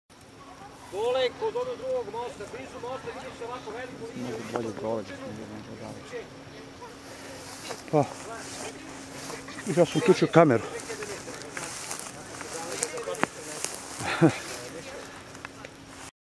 2012-09-29: Трчање у организацији АК Јасеница (2. део)
Атлетски клуб „Јасеница" је у суботу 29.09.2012, организовао сат времена трчања, шетње, релаксације, дружења, ...
На реци Јасеница окупљање је било код малог (плавог) моста од 17 ч. Акција се наставља сваког викенда.